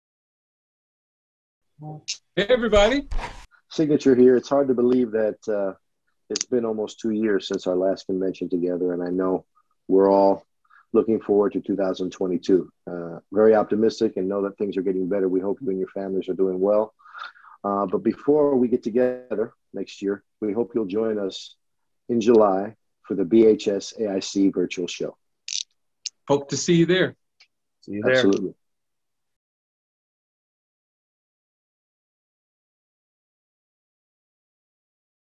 2019 International Quartet Champion Signature invites you to Virtual International!
Whether you want to see the AIC Show, the chorus festival, partake in the nightly afterglows or all of the above, you do NOT want to miss our Virtual International event! Register here: